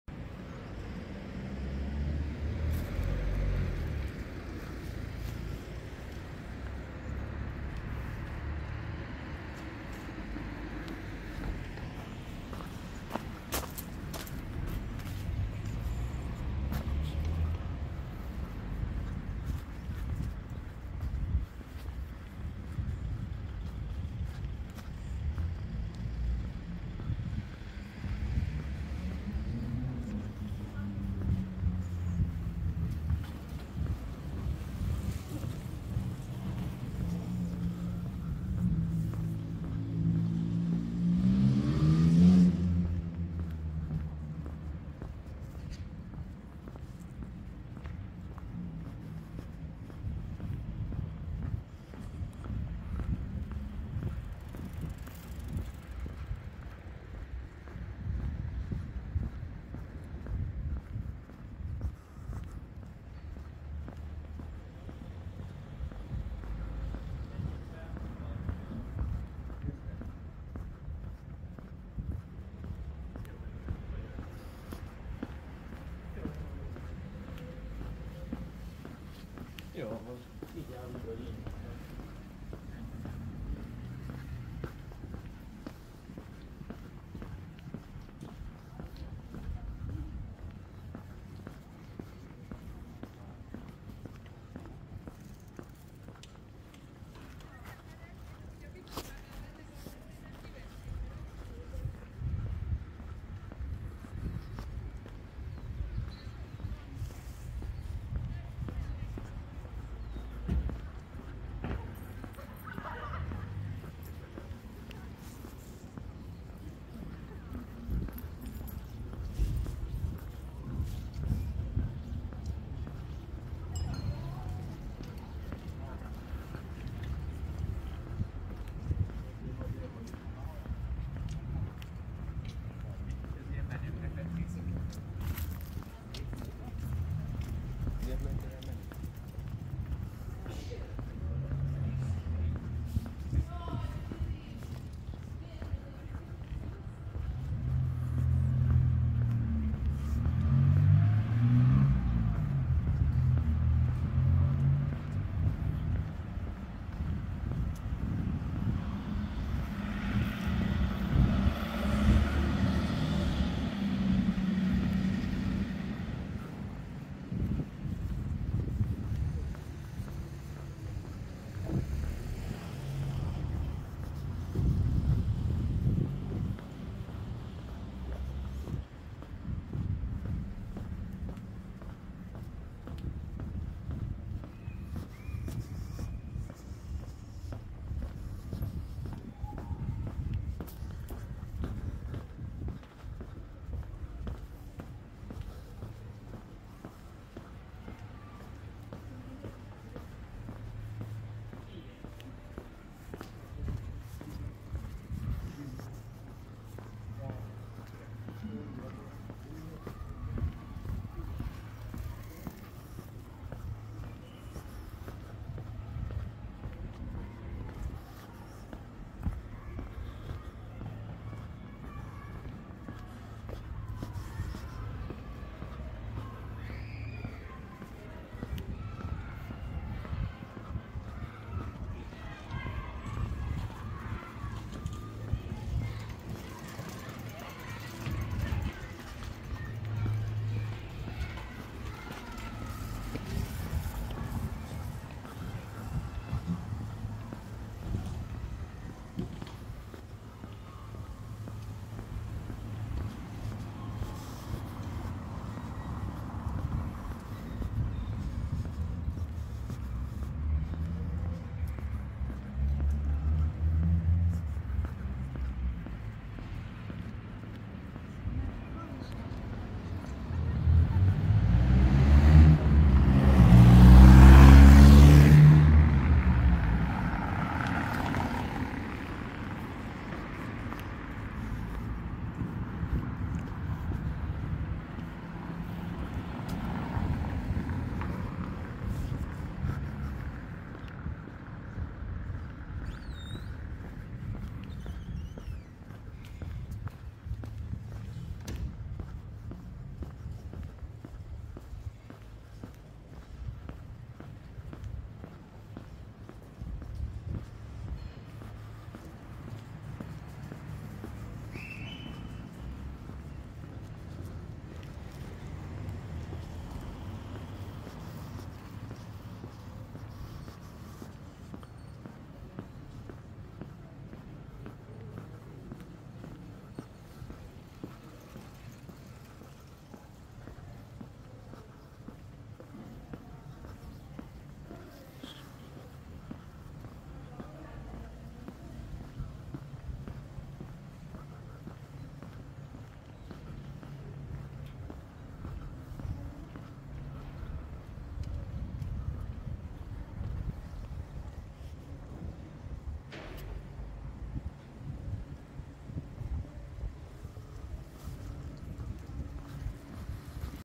Békéscsaba Walk 4K - Capa Söröző to Speed Burger
Thursday 16/11/23
1pm
13C
55F

This short walk takes us down the pedestrian and bicycle street of Csaba ut, from Capa Söröző, my favourite bar in the Békéscsaba town center, to Speed Burger, on Justh Gyula utca, home of the best burgers in Békéscsaba